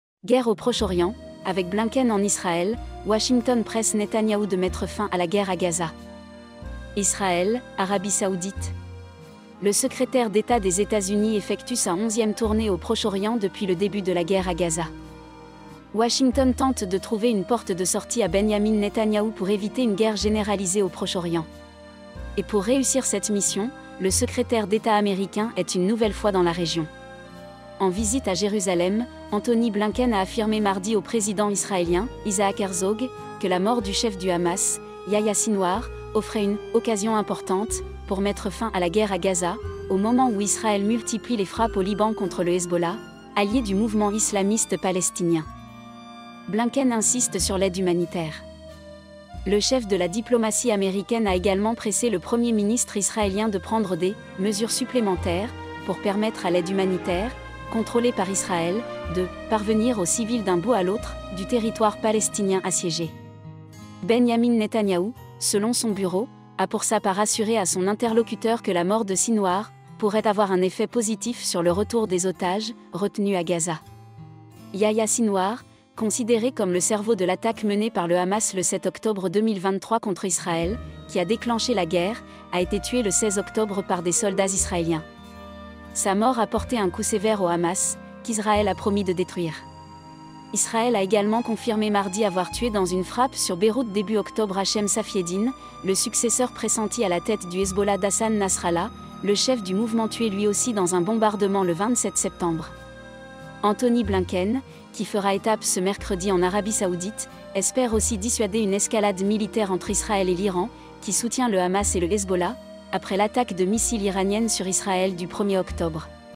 Guerre au Proche Orient Avec Blinken en Israël, Washington presse Netanyahou de mettre fin à la gue
Israël, Arabie saoudite… Le secrétaire d’Etat des Etats-Unis effectue sa 11e tournée au Proche-Orient depuis le début de la guerre à Gaza. From 20minutes
MAKE SURE TO FOLLOW ON MY VIDEOS AT SANPISETH.NET